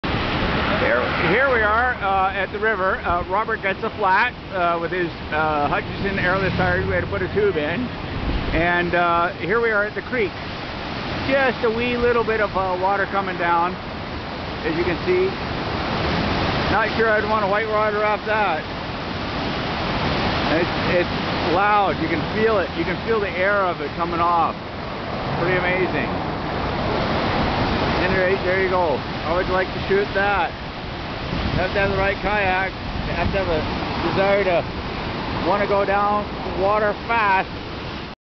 Boulder Canyon decent
Robert got a flat on the decent and along side the road is the creek from the dam, as you can see the water is at it's highest in the last 20 years